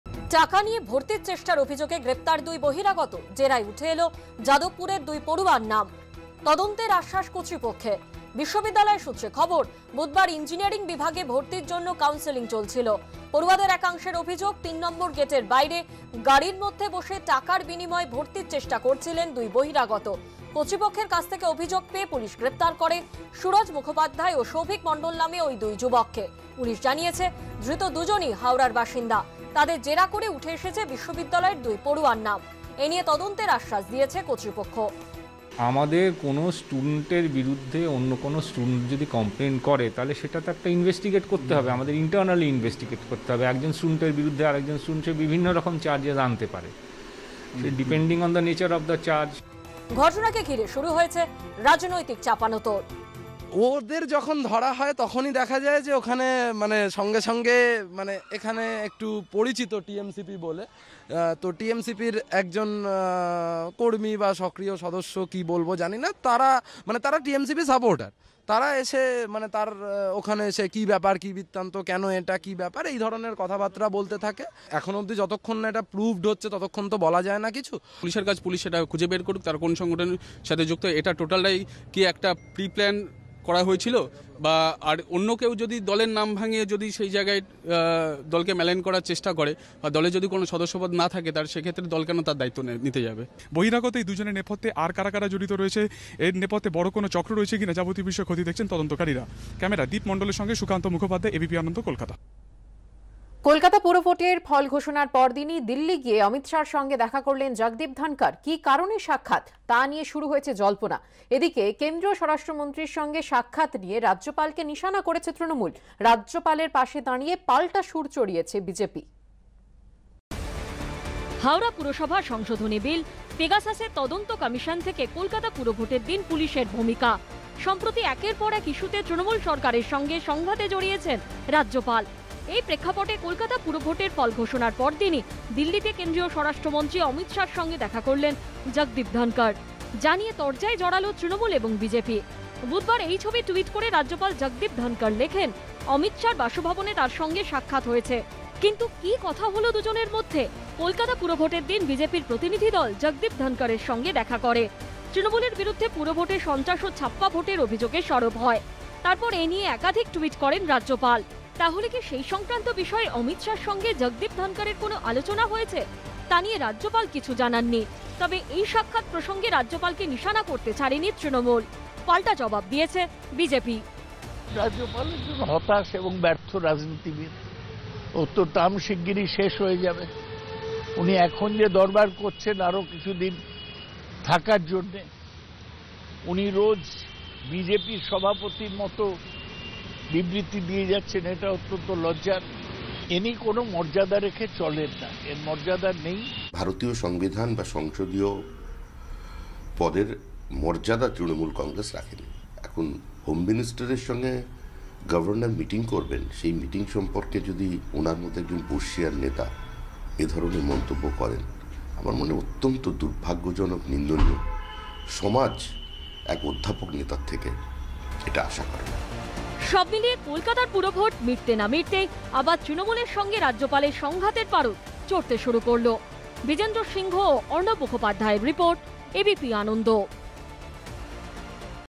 JU: যাদবপুর বিশ্ববিদ্যালয়ে ইঞ্জিনিয়ারিং বিভাগে টাকা নিয়ে ভর্তির চেষ্টার অভিযোগ, গ্রেফতার ২
যাদবপুর বিশ্ববিদ্যালয়ে (Jadavpur University) ইঞ্জিনিয়ারিং বিভাগে টাকা নিয়ে ভর্তির চেষ্টার অভিযোগ। ২ যুবককে গ্রেফতার করল পুলিশ। বিশ্ববিদ্যালয় সূত্রে খবর, ধৃতদের জেরায় উঠে এসেছে বিশ্ববিদ্যালয়ের ২ পড়ুয়ার নাম। তাঁদের ভূমিকা নিয়ে তদন্তের আশ্বাস।